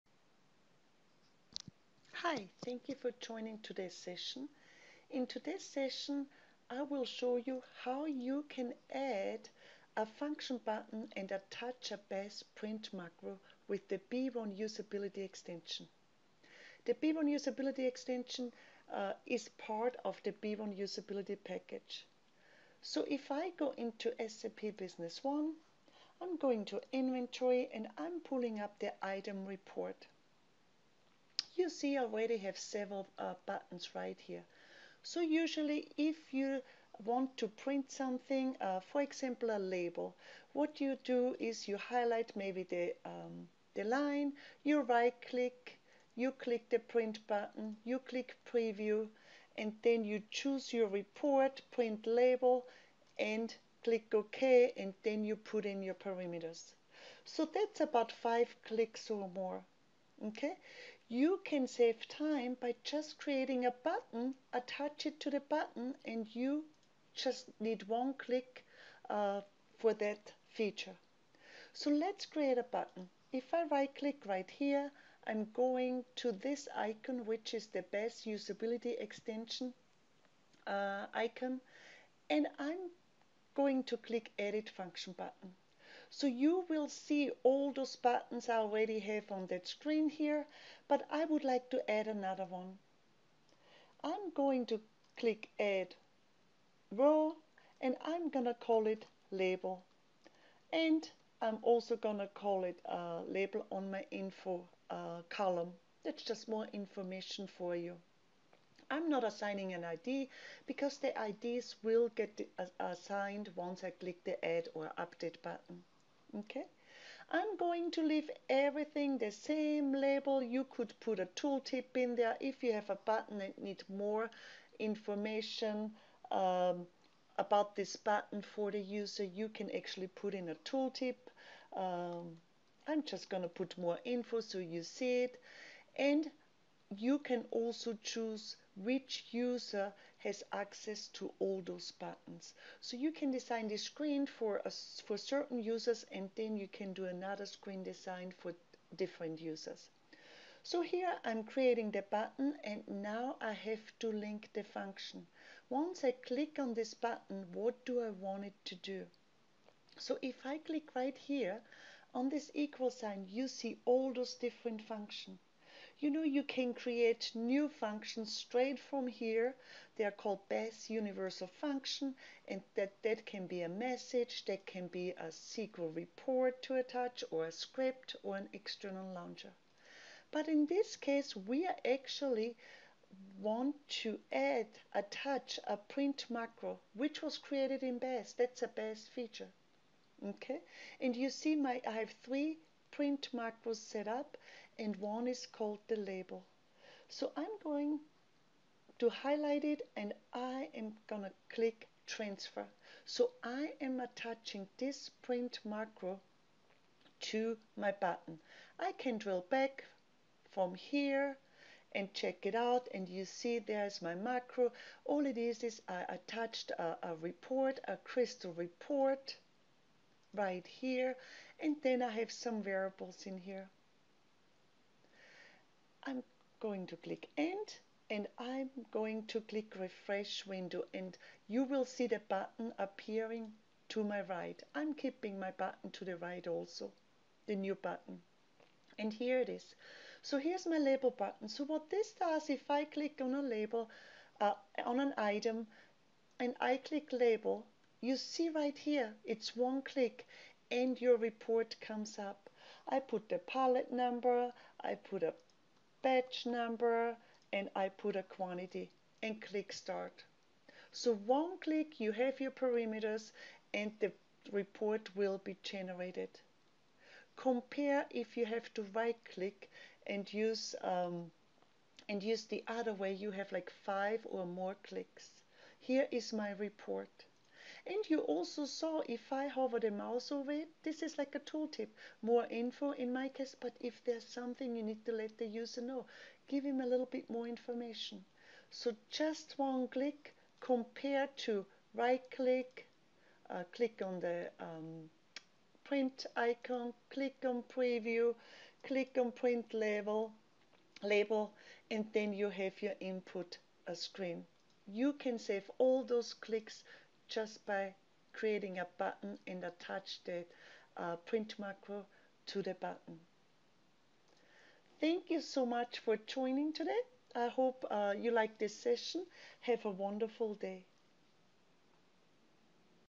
How to create a Print Macro button
In this video we will show you how to link a Beas Manufacturing print macro to a function button to save time.

SImple customization for Beas Manufacturing with B1 Usability Package